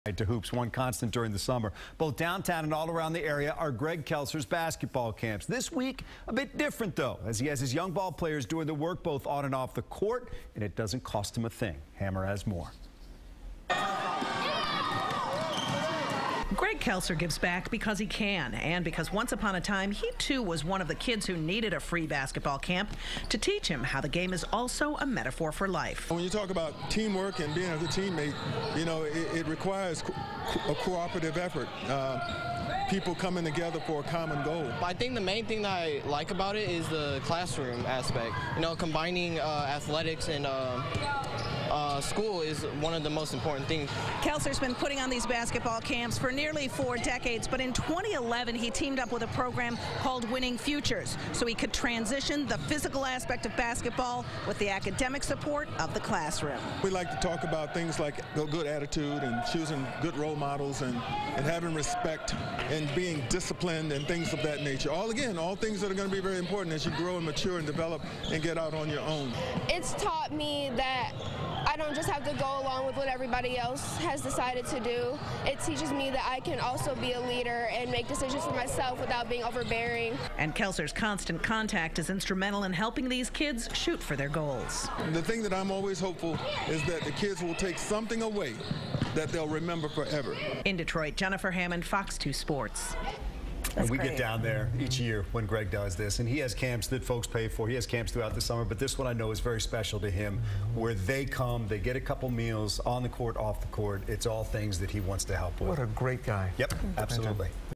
Greg Kelser Basketball Camp helps student-athletes on and off the court
Former Michigan State University star and current FOX Sports Detroit Pistons analyst is hosting his annual summer basketball camp